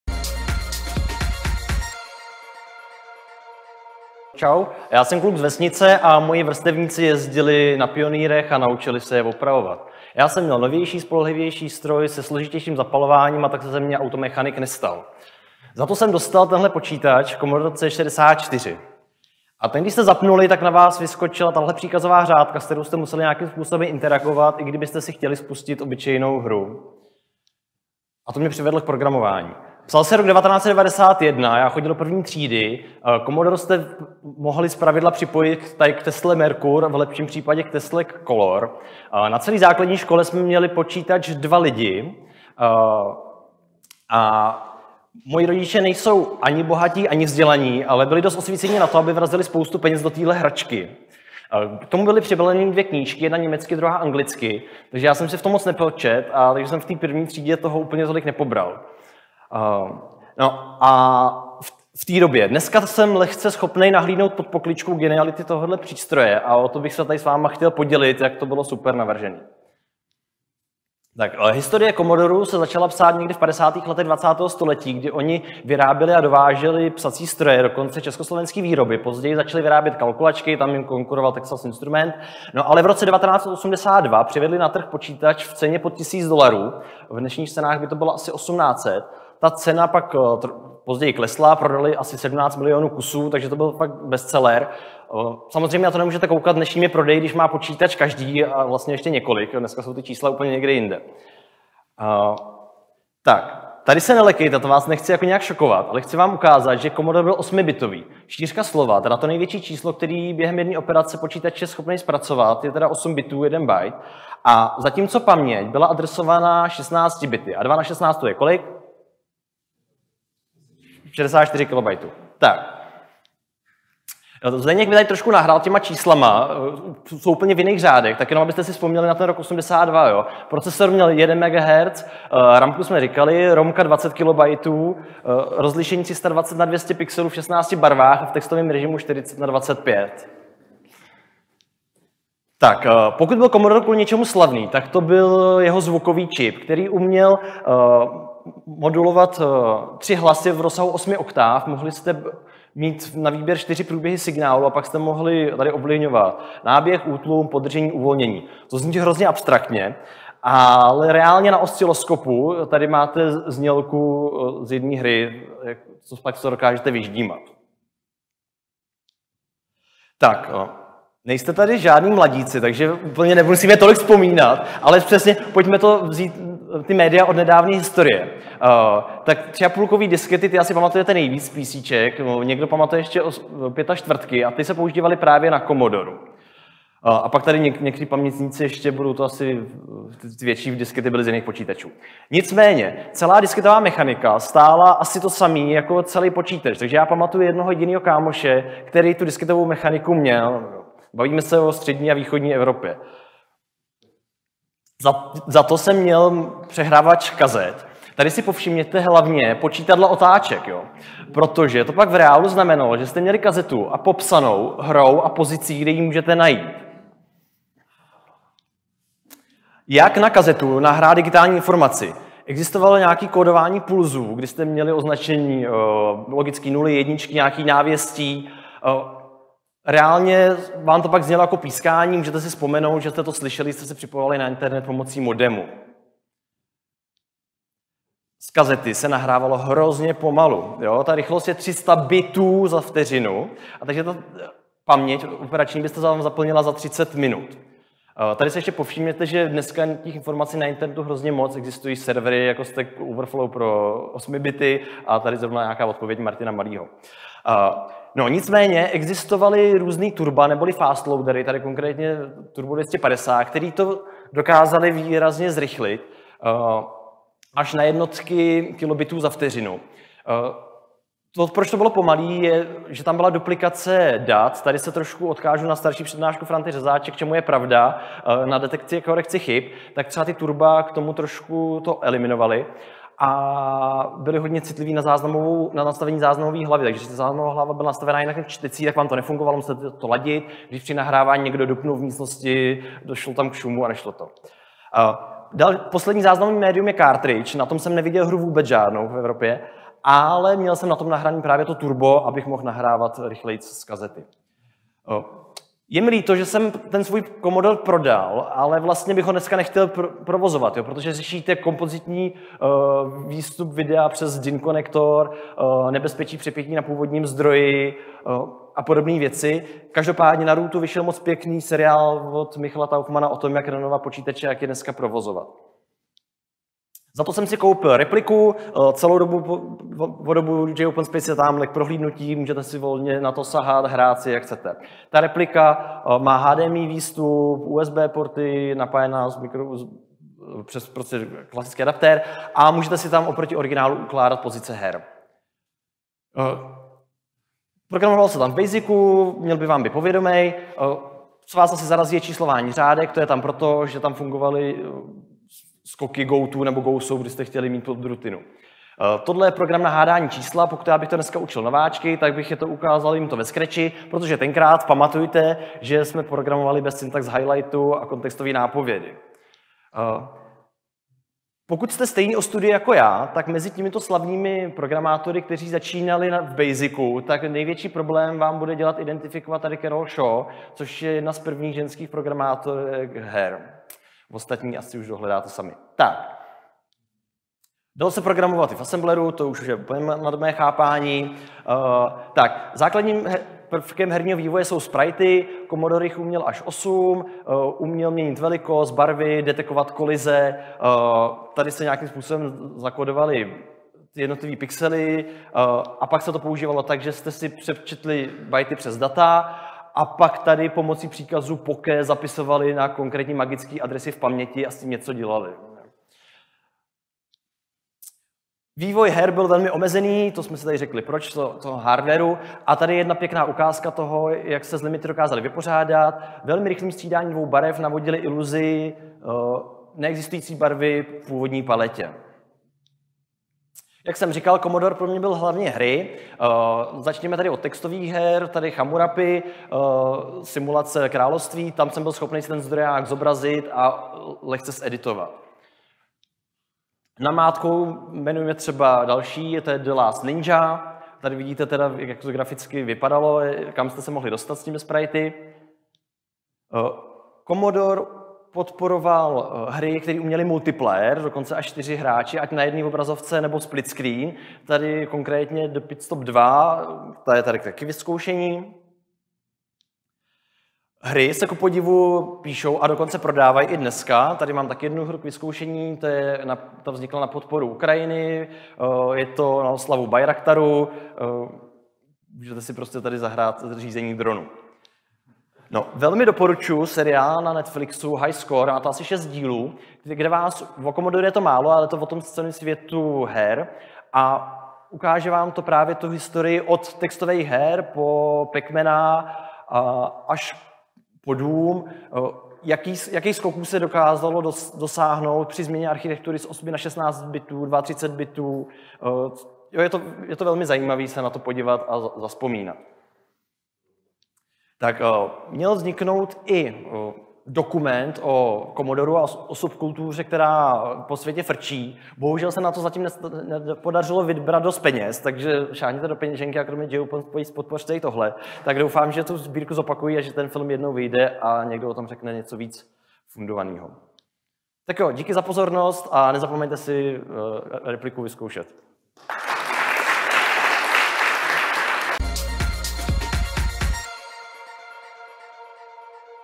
jOpenSpace 2023 - Luboš Račanský - Pocta C64
- Netflix - doporučen seriál High score
- I Adore My 64 - crowdfunding akce pro natočení filmu o C64